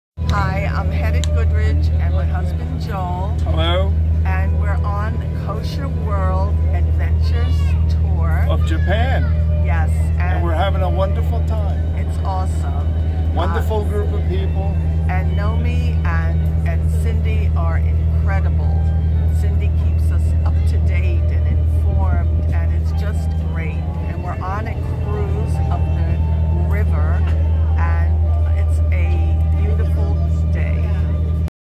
Hedda and Joel loved Kosher Japan with Josh Rubin and Kosher World Adventures August 2024
A Tour that allows you to keep Glatt Kosher and Daven with a minyan!

Very High Level of Kashrut, Tefilah, Sefer Torah and Shmirat Shabbat

*Learn about Shoguns, Samurais and Ninjas

*Experience a traditional Japanese tea ceremony.

*Owakudani Active Volcano - Cable car ride

*Experience an Authentic Japanese Karaoke Club

*Emotionally connect - Hiroshima and the site of the A-Bomb dome

*Boat cruise along the Ashi River with views of Mount Fuji!

*Experience the serenity of Japanese Nunobiki Gardens

*Be amazed at the Unique Japanese Artistry.

*Sushi and Japanese Caligraphy Workshops